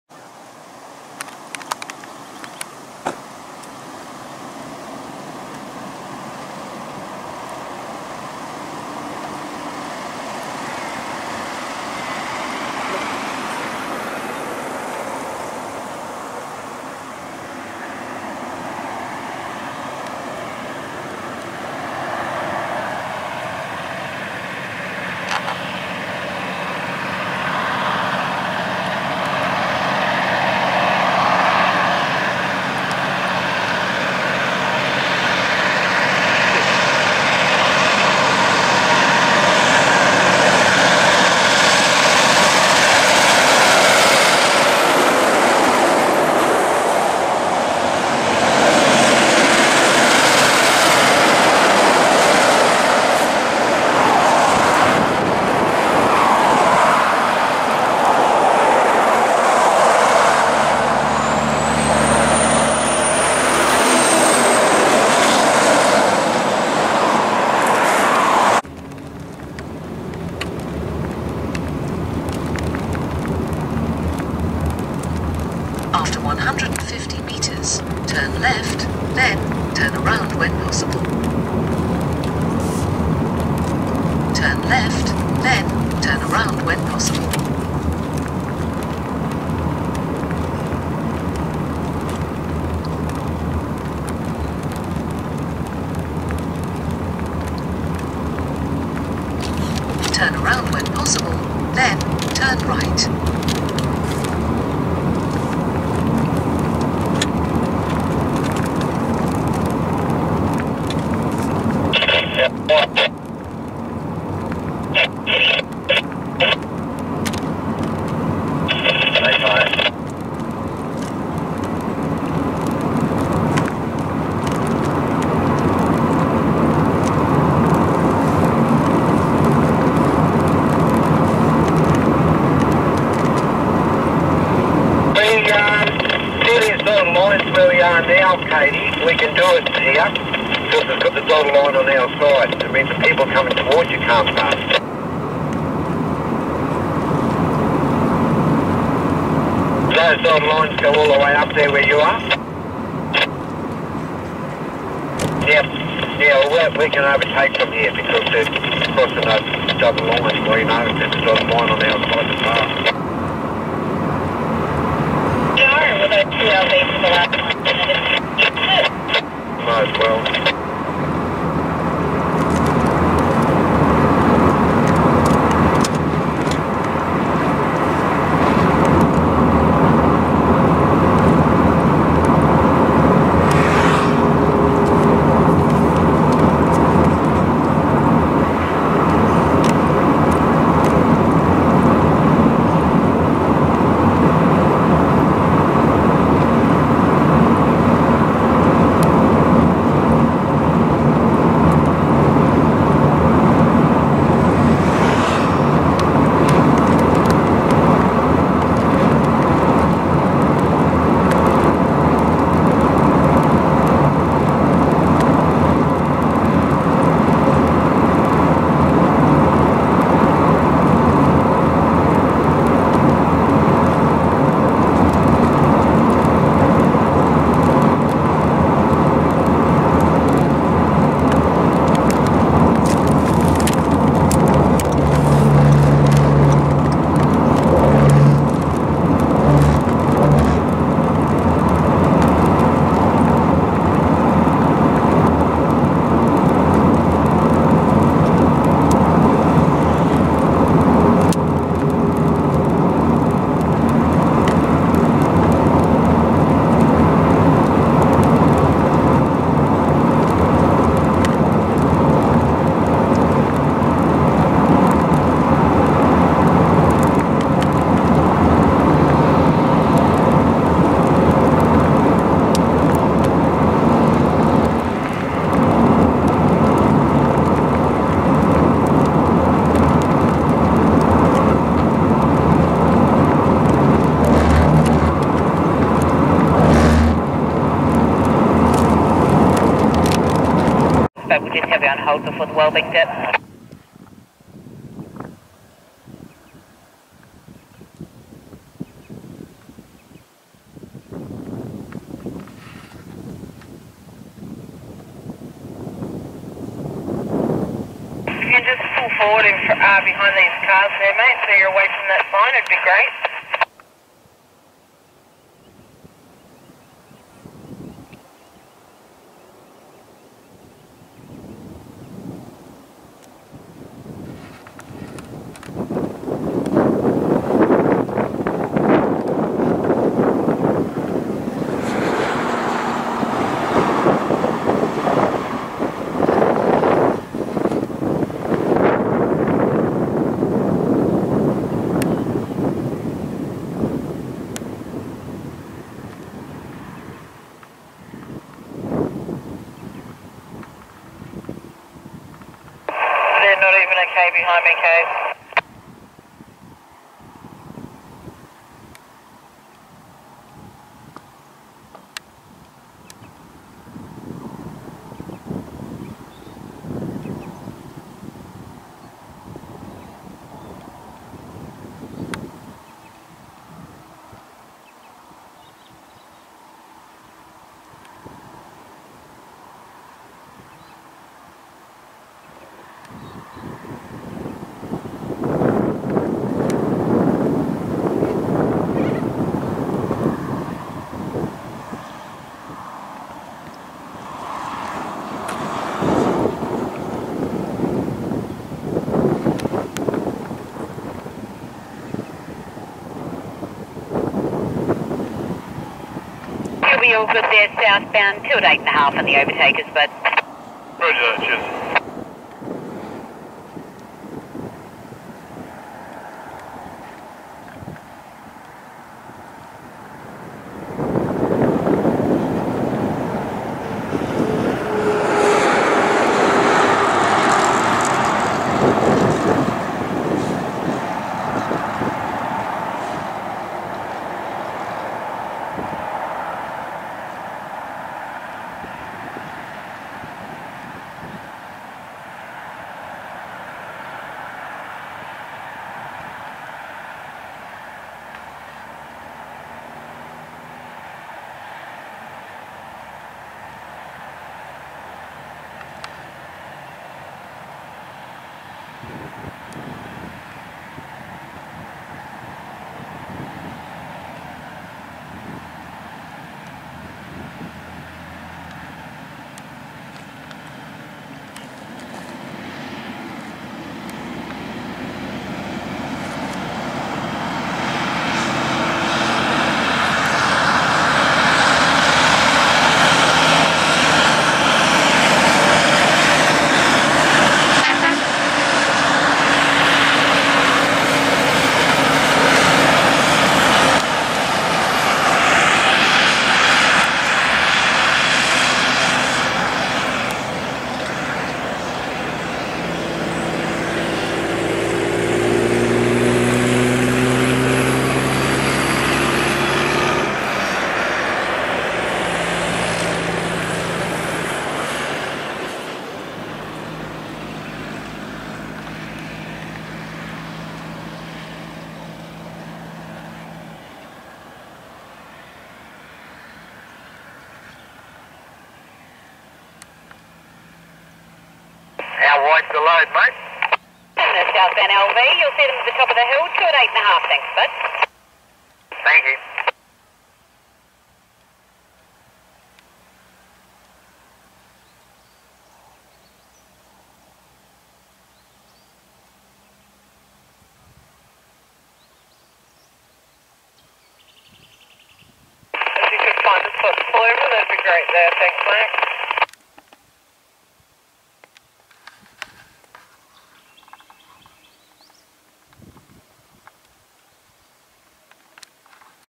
🚛. Centurion Transport Kenworth K200 x 2. With Hitachi EX 3600 mining excavator in two parts
Two of Centurion Transport's Kenworth K200's are hauling the chassis body and undercarriage in two 8.5m width loads north on Great Northern Highway.
I was heading south, when these guys turned up. The helper trucks were no longer needed now.
I grabbed a passing video clip here, and then turned around to chase them up. for a couple more scenes.
2nd location. at Waddington,  overtaking the heavy oversize convoy.
3rd location, as viewed from the Walebing curve.